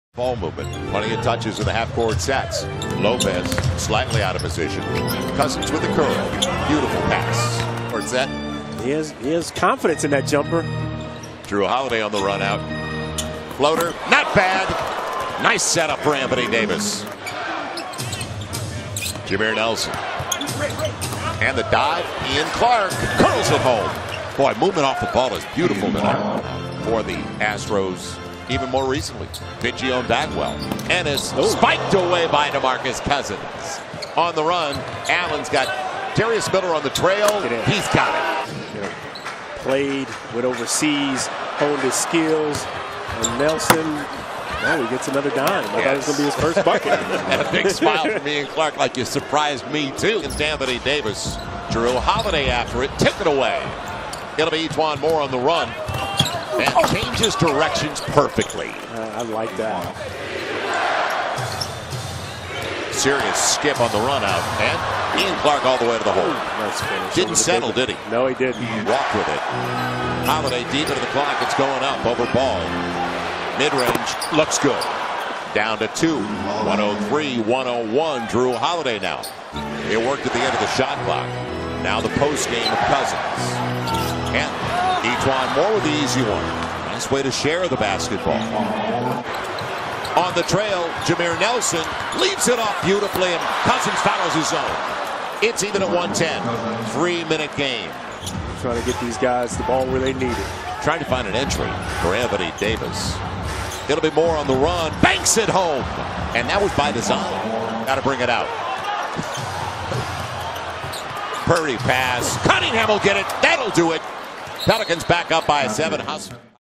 Game Highlights: Pelicans vs Lakers 10/22/17
Pelicans get the win over the Los Angeles Lakers at the Staples Center.